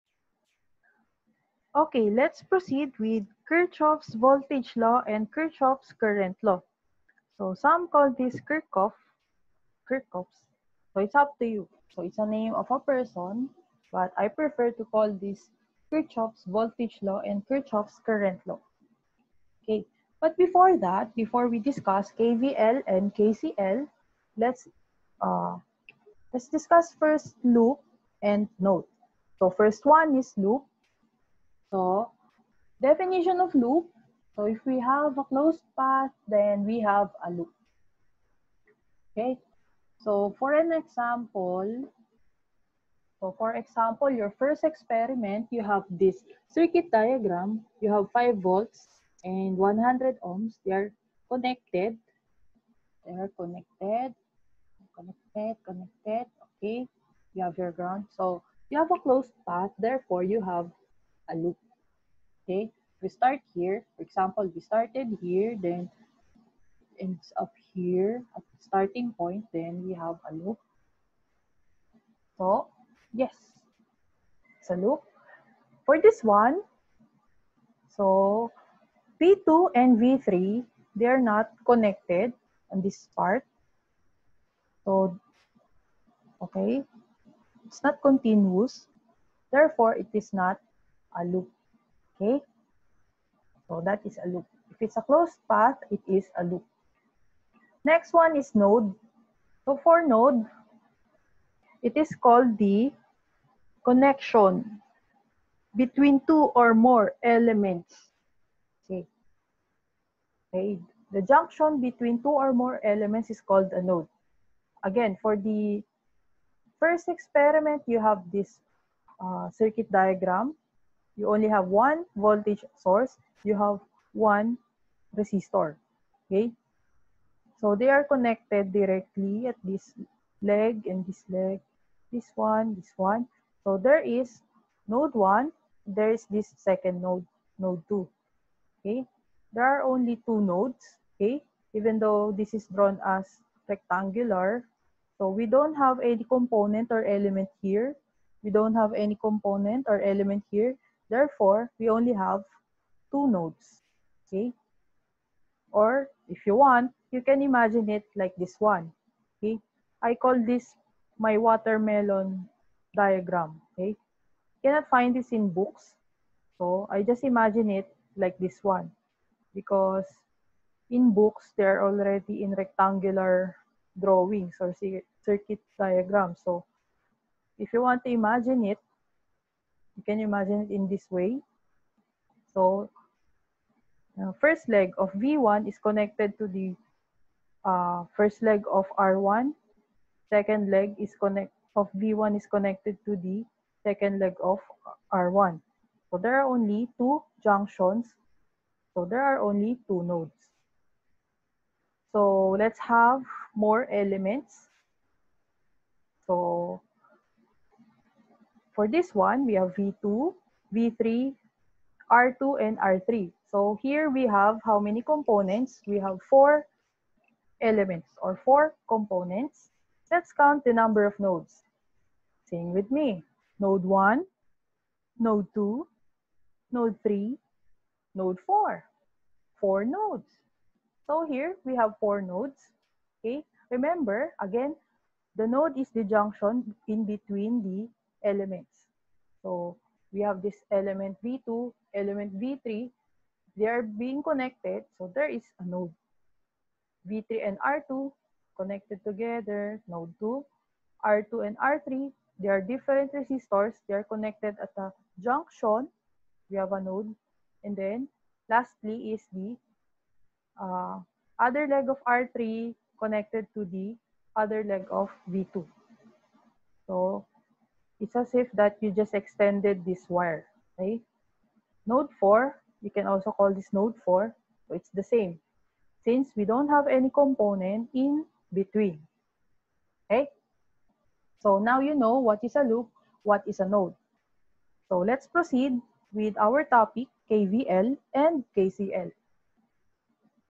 What is KVL and KCL? Part 1
Discussion of Loops and Nodes prior Kirchhoff's Voltage and Current Law